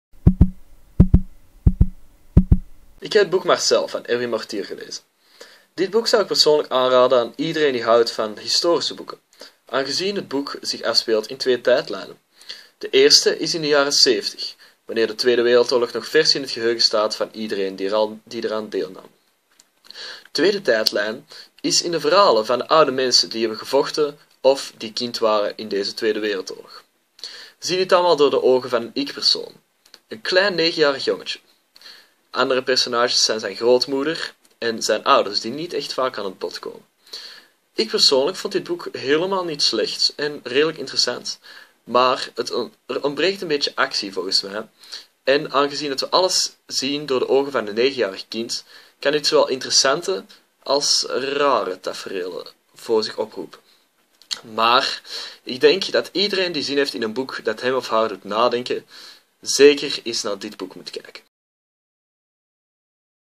Seba over Marcel - Erwin Mortier
– BOEKENTIPS IN 1 MINUUT – "Ik denk dat iedereen die zin heeft in een boek dat hem of haar doet nadenken zeker eens naar dit boek moet kijken." – Sebastiaan, , student, Mortsel (België).